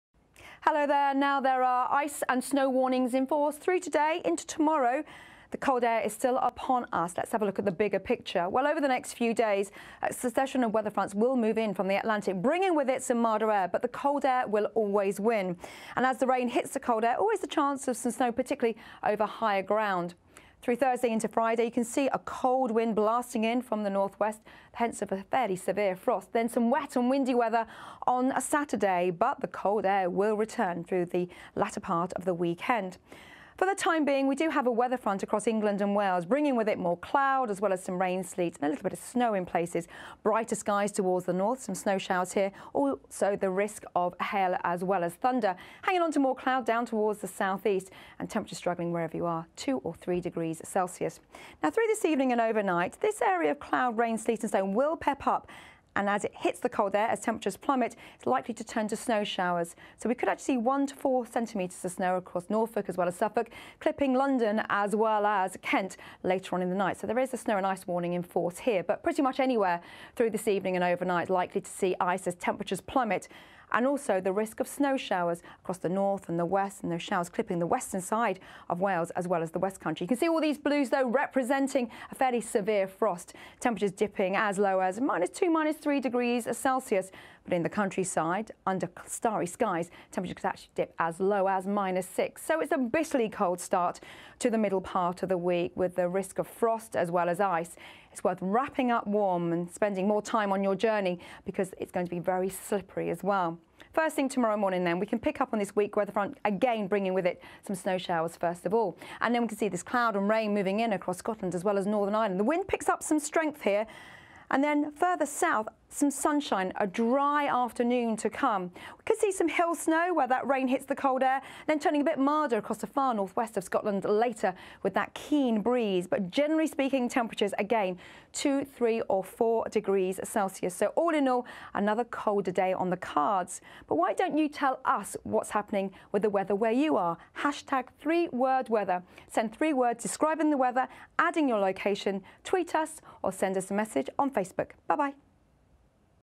UK: Snow & ice warnings
Sleet and snow showers will continue to affect parts of eastern and southeastern England later on Tuesday, lasting well into the night in places. Some mostly small accumulations of snow are likely. This is an update to focus the warning area further south. Expect some delays to travel, while untreated surfaces become icy, leading to an increased risk of accidents or injuries.

Chief Forecaster's assessment

A band of sleet and snow will turn more showery overnight but with some heavier bursts possible, especially over Norfolk and Suffolk. 1-4 cm of snow is possible in places there, though rain and sleet may occur along coasts. Elsewhere some places seeing a cm or so of snow but ice may be the more widespread hazard.